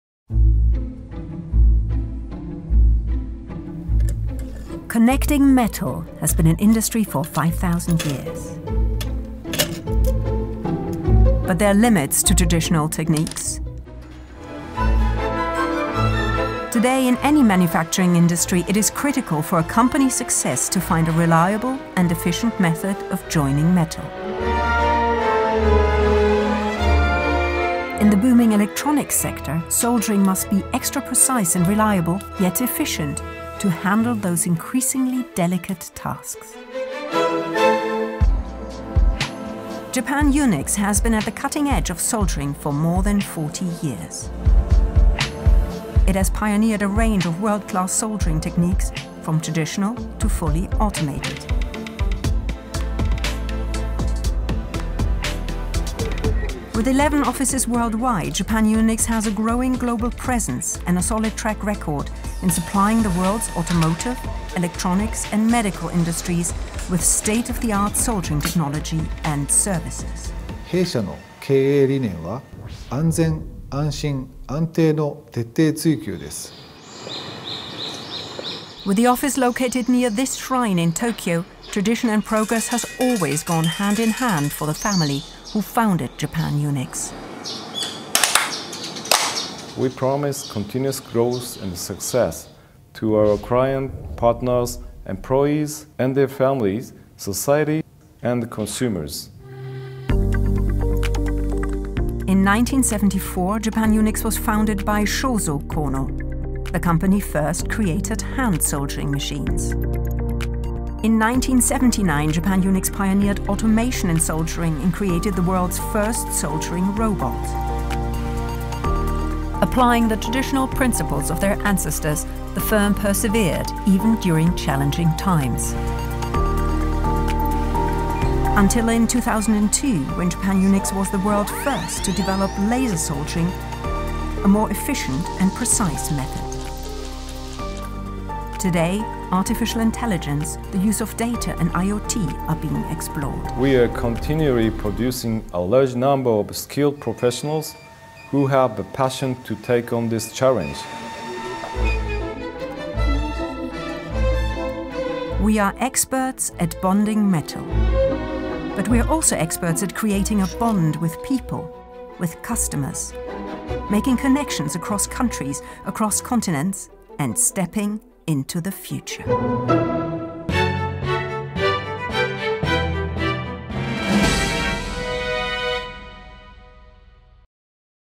Japan Unix lehimleme robotları firması tanıtım videosu
Japan Unix lehimleme robotu (robot lehimleme) ve lazer lehimleme tarihini geliştiren küresel lehimleme endüstrisinin lider şirketidir.
Film, şirketin son 45 yılda nasıl kurulduğunu ve büyüdüğünü tanıtıyor. Bu, 1970'lerin sonunda geliştirilen dünyanın ilk lehimleme robotunu sunar.
Bu video, müşteriler, tüketiciler ve tüm paydaşlar için robotik lehimleme teknolojisinin gelişimini anlatıyor.